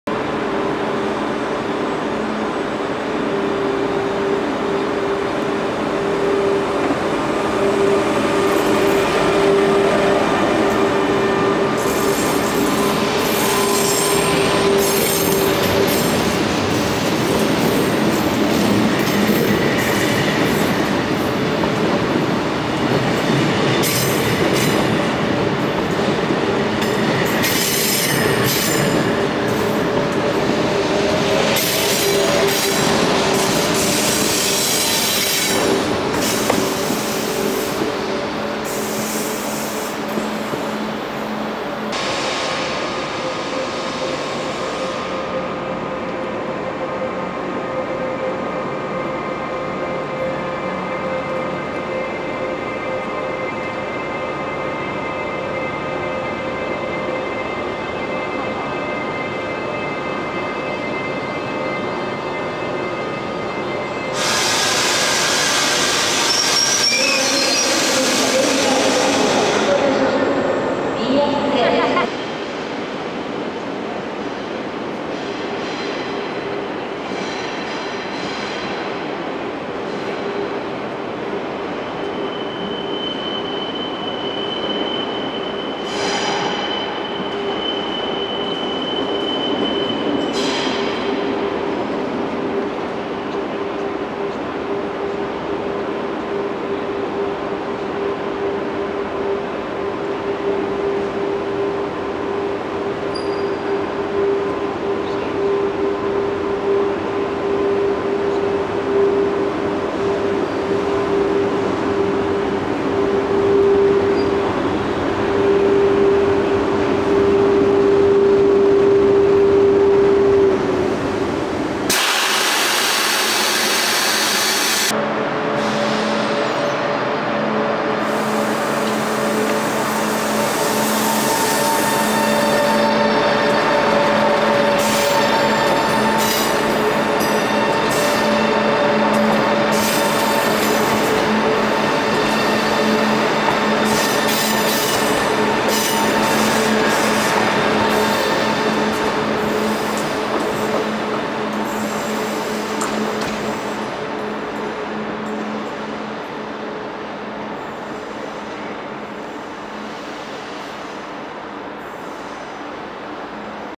Moviment a l'Estació de França
1. R2 Barcelona EdF - Vilanova i la geltrú
2. Talgo Garcia Lorca Sevilla Santa Justa - Barcelona EdF
3. R2 Sant Vicenç de Calders - Barcelona EdF
4. Material buit Barcelona EdF - Barcelona Sants